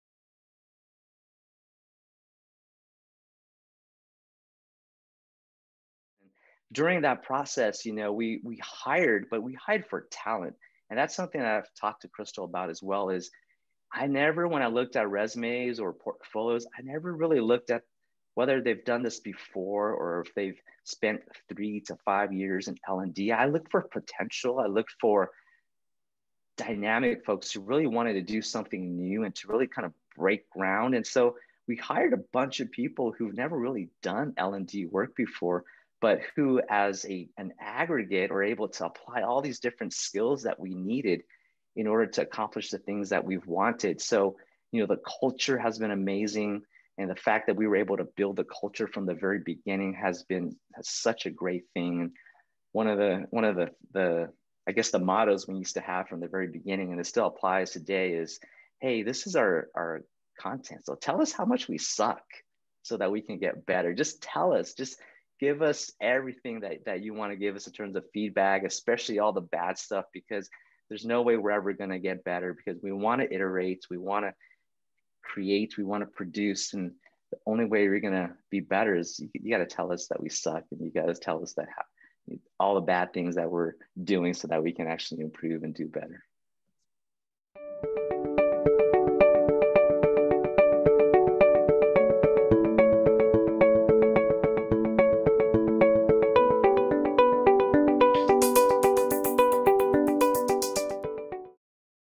How to Build a Modern Training Team from Scratch
Loc Nguyen, Director of L&D at Bluescape, shares how he built a team of L&D professionals, capable to design modern learning and especially support the vision of Learning Clusters (rather than courses or classes).